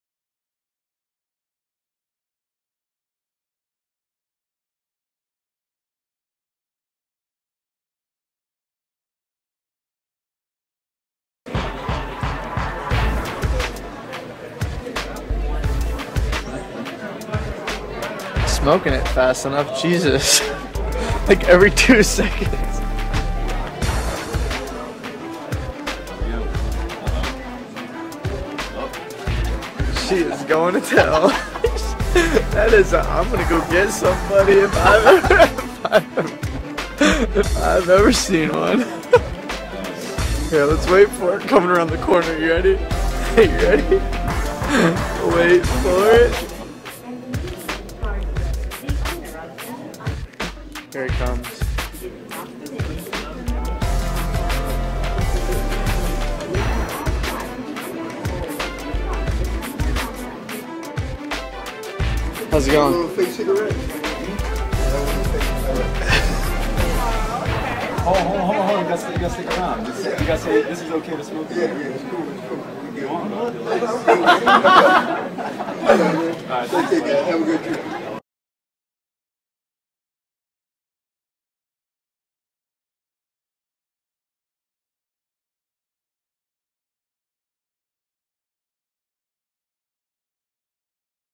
Electronic Cigarette - Smoke in Terminal - SKY Cig
This is a video of some guys smoking electronic cigarettes in an airport terminal and getting the "OK" from security and police.